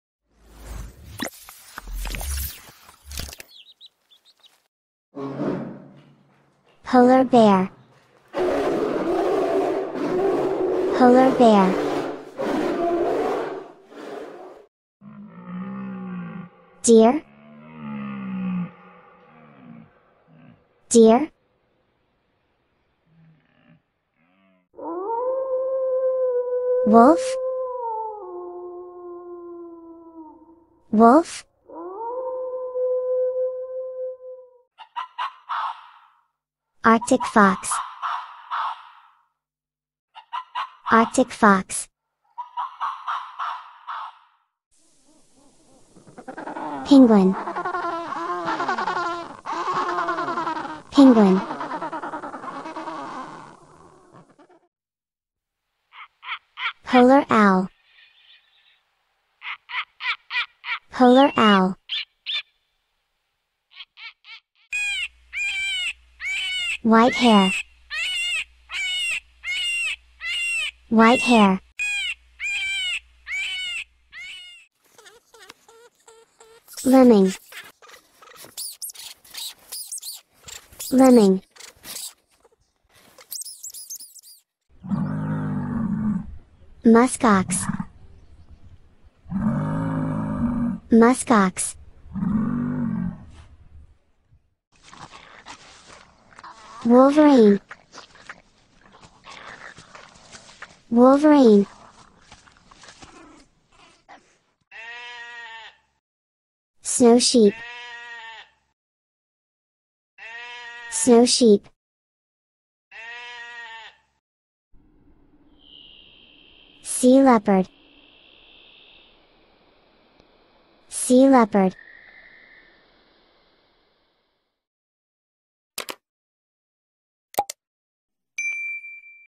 Animals of the North and their sounds. Animals of the Arctic. Educational video for children.
In this exciting video, children will get acquainted with animals living in the north: polar bear, penguin, reindeer, arctic fox, polar wolf, wolverine, snowy owl, sea leopard, arctic hare, musk ox, lemming, bighorn sheep. Children will hear the sounds of these animals. This educational video is very exciting and useful for the development of children.